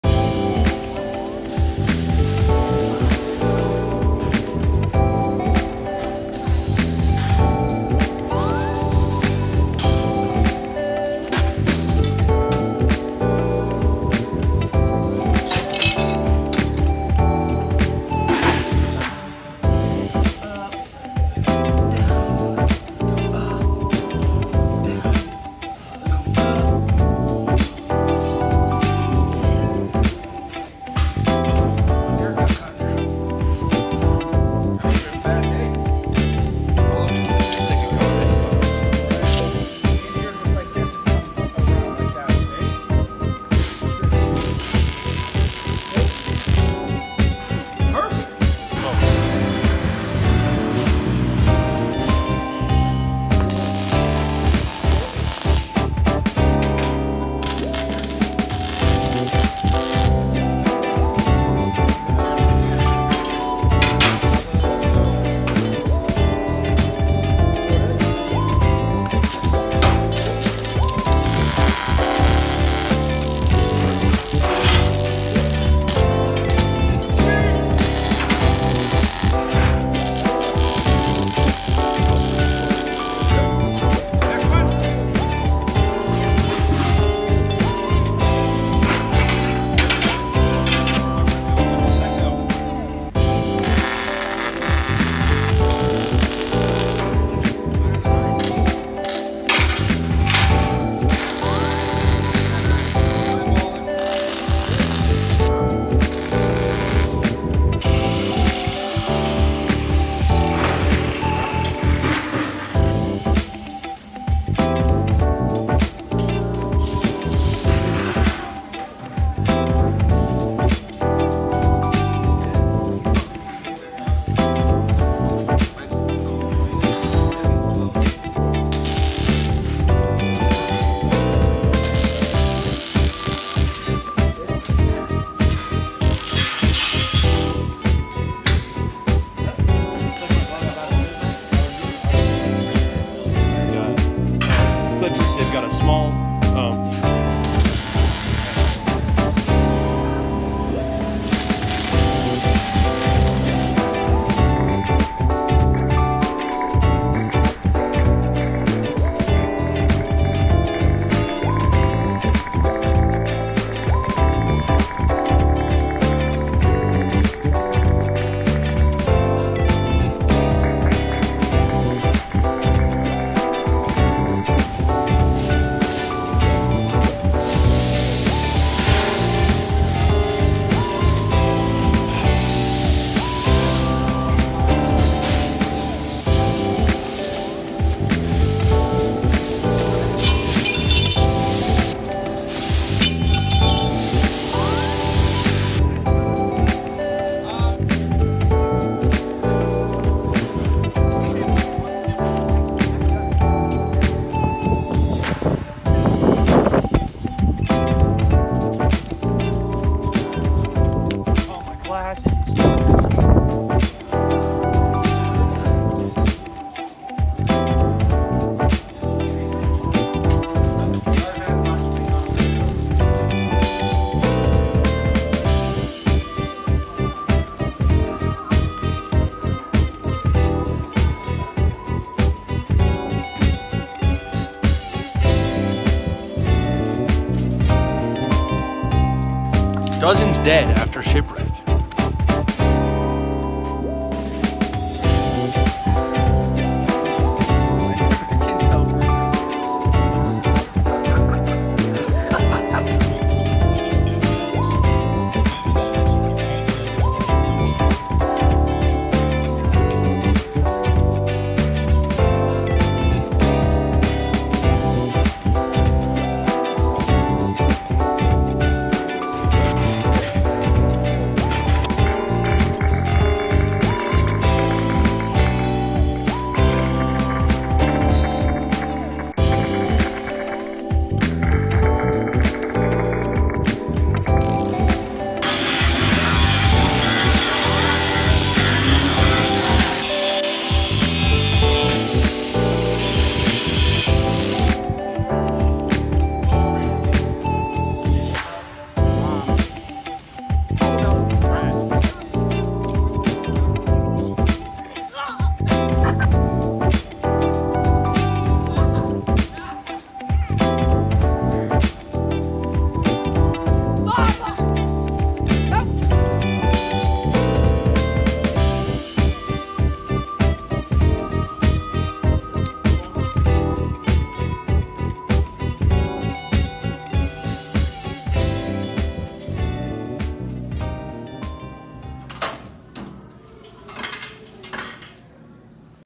CSI Engineering Industry Career Camp (Day 4) NERD CAMP
Today we welded in clays shop. We made wind thingys and learned to mug weld. Lunch was pretty mid today but they were watching my videos so that’s kinda cool